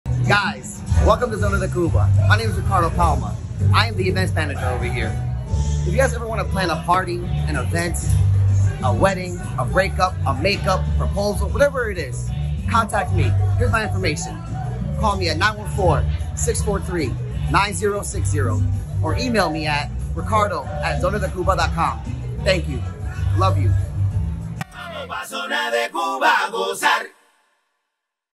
Come and celebrate your special occasion at Zona de Cuba
Ricardo Palma, Events Manager of Zona de Cuba invites people to celebrate their special parties at Zona de Cuba